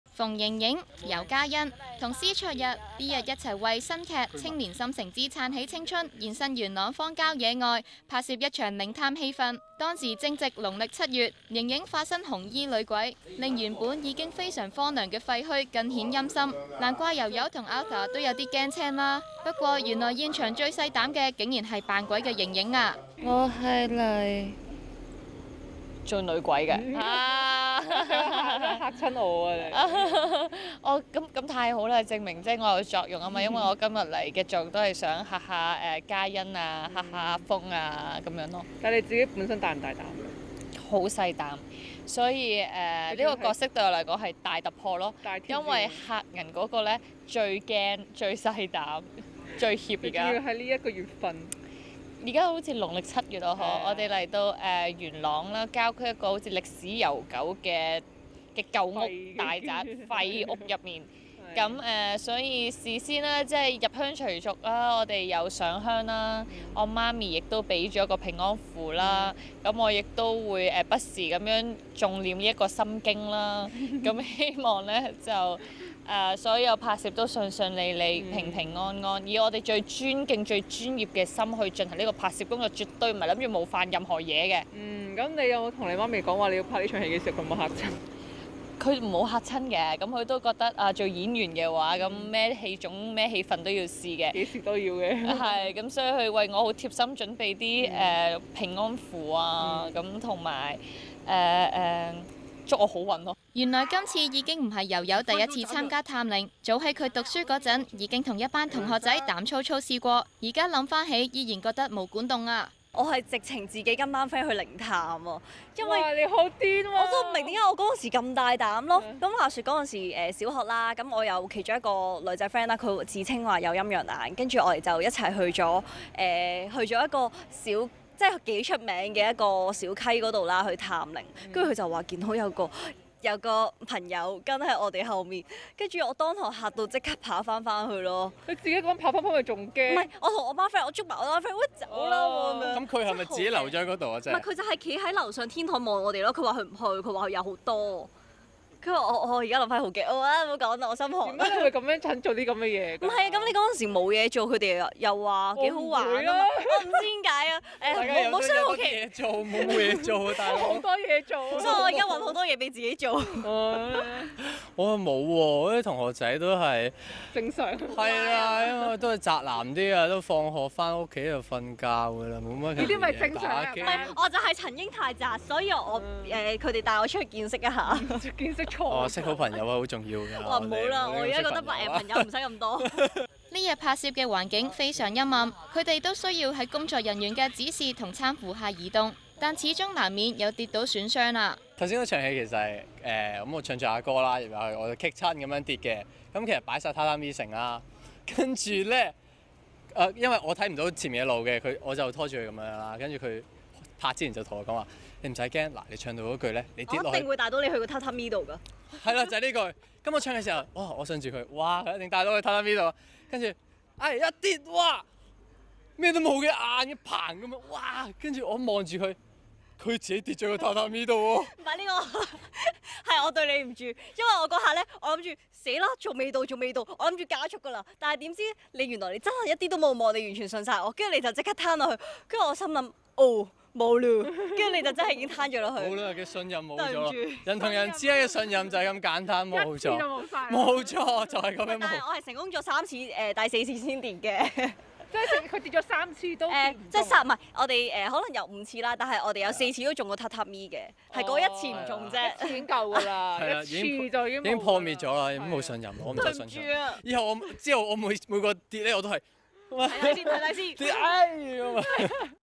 娛樂新聞台｜馮盈盈｜農曆七月化身紅衣女鬼
【娛樂新聞台】馮盈盈農曆七月化身紅衣女鬼
馮盈盈化身紅衣女鬼去嚇人？原來佢早前為《青年心城之撐起青春》拍攝一場探靈戲，佢就扮女鬼去嚇游嘉欣同埋施焯日。要嚇人嘅盈盈坦然話自己好細膽，拍攝時仲係農曆七月深夜荒郊野外，更加令佢提心吊膽，而盈盈媽咪仲求了一道平安符俾佢傍身。

Like + Follow = 緊貼 TVB 各劇集及藝員動態！